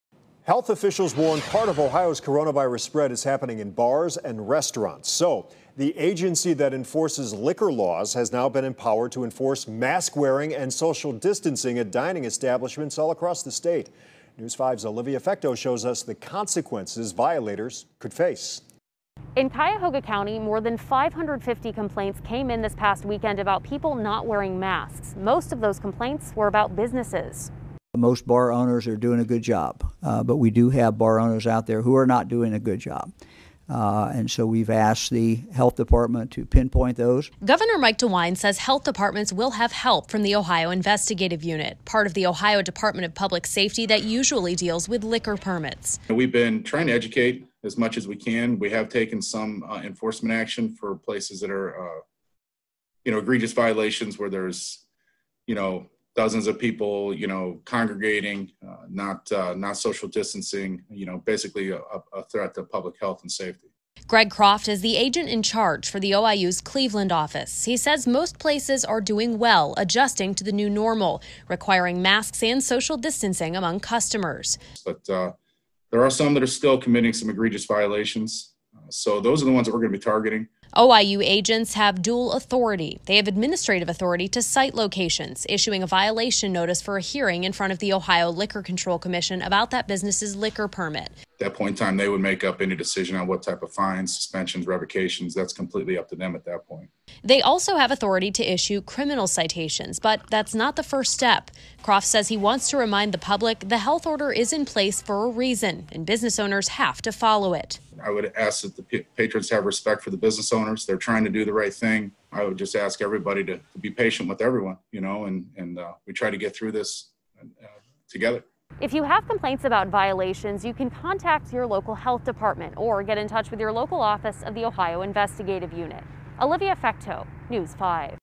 Enforcing Ohio's mask wearing and social distancing guidelines at bars, restaurants
Governor Mike DeWine announced Thursday that the Ohio Investigative Unit, a division of the Ohio Department of Public Safety, would be assisting in enforcing rules for mask-wearing and social distancing at bars and restaurants.